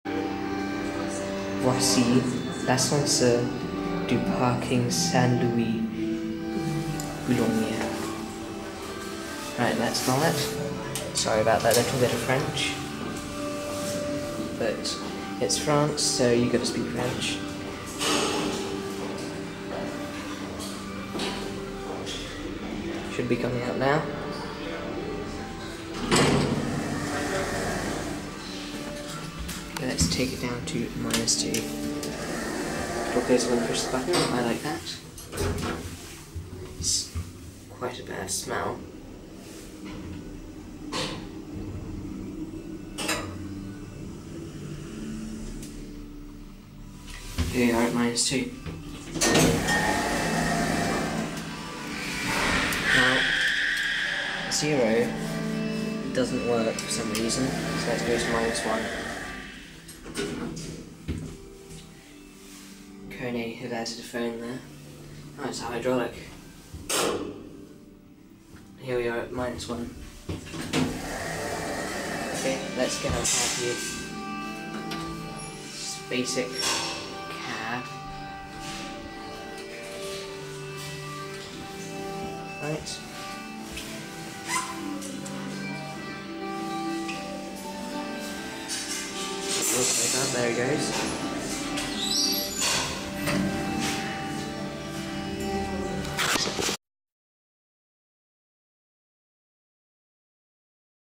Guinet elevator at St Louis Car Park, Boulogne-sur-Mer
I promise that I will only be speaking French for this video! The first of my videos from France is of an elevator from a French company, in a very nice car park (compared to the ones in England!)